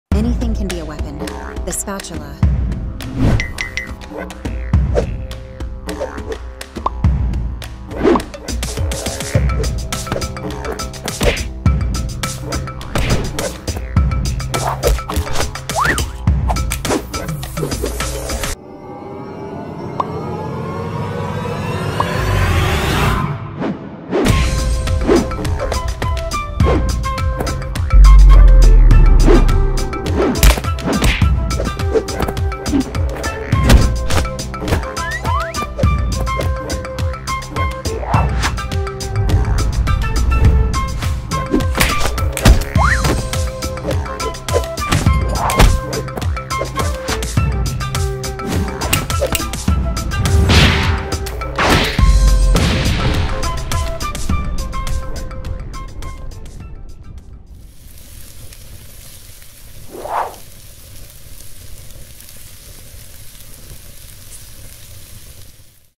Anything Can Be A Weapon - 1
Animation by suprsoup made in Pivot Animator. A fun idea that originated in my head and ended up being actualized into a real video. Hope you enjoy!

Anything Can Be A Weapon! Or can it... this episode: The spatula! Comment what unconventional item you want to see used as a weapon next!

Subs at 5/5/2024: 44.7K! Subs are disappearing everyday, let's goooo!